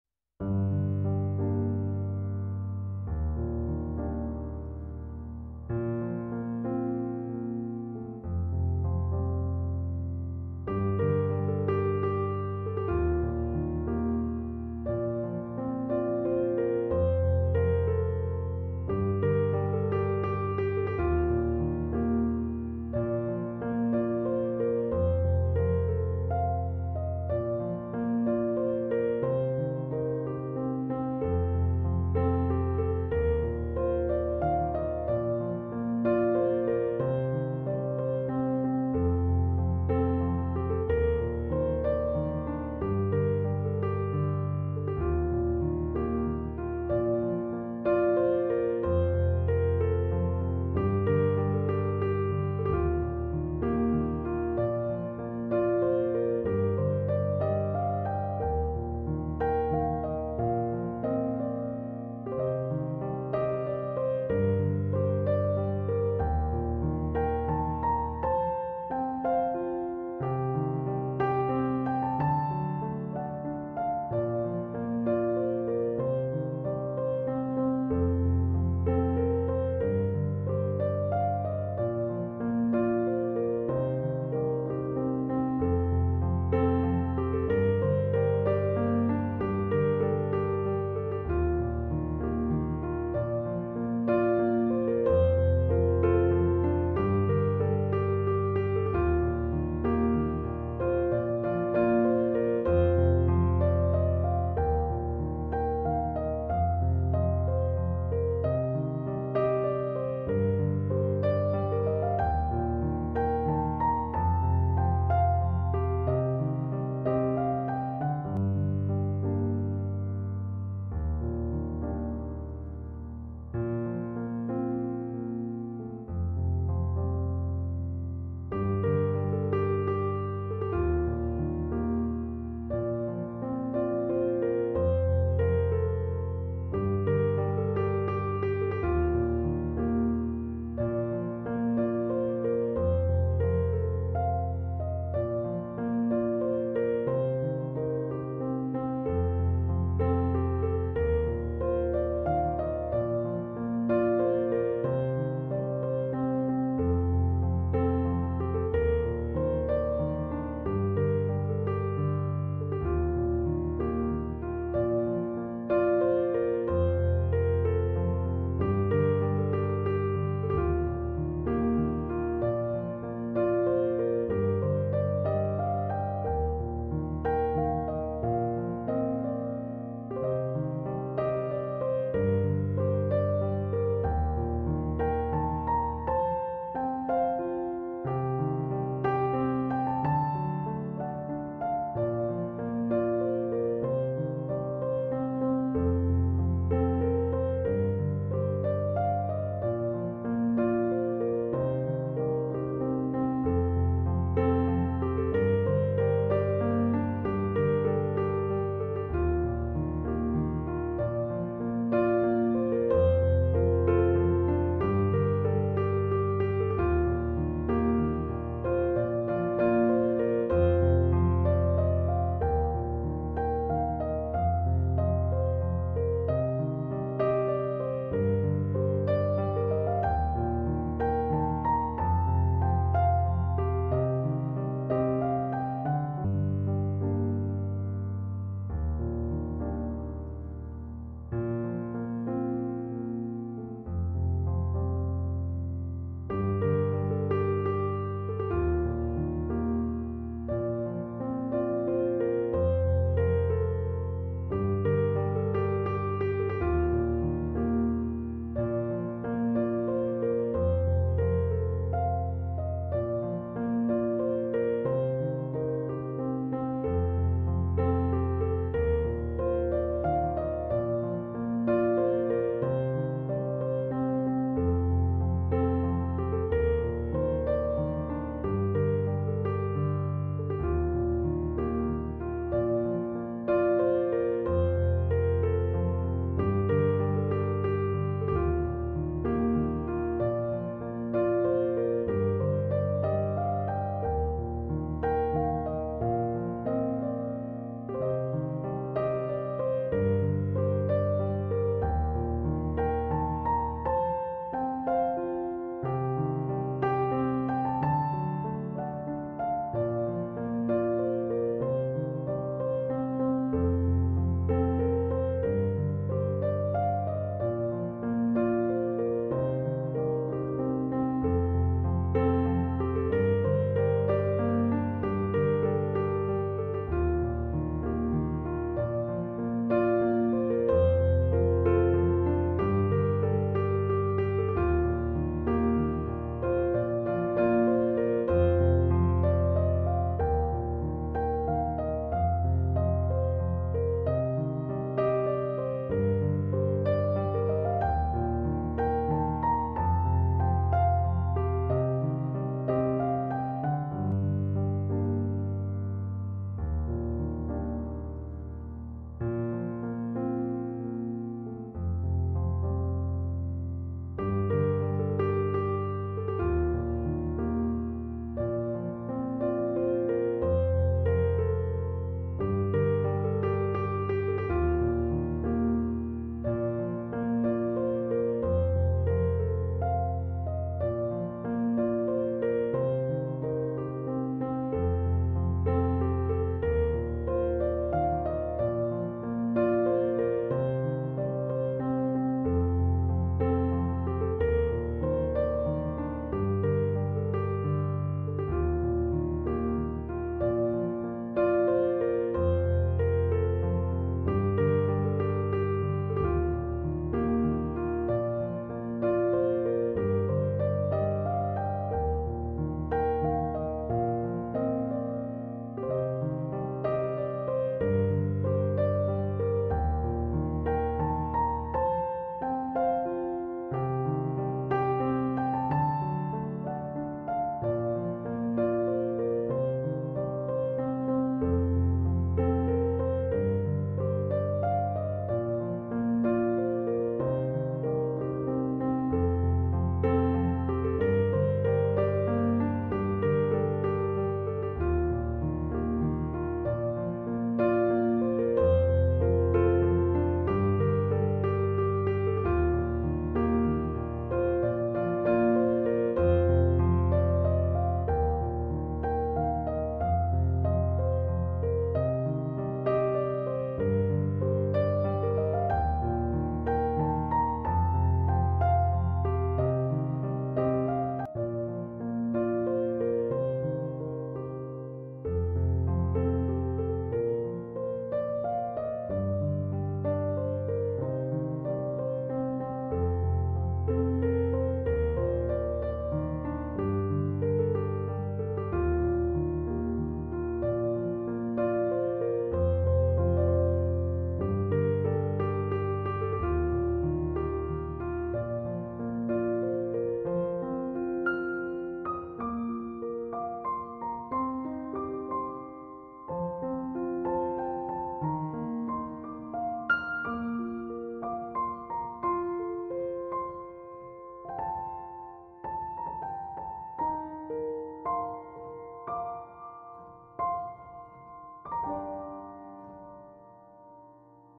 Relaxing music || What you need for peace of mind
Calm and relaxing music, with a beautiful illustration, will help you recover after a working day.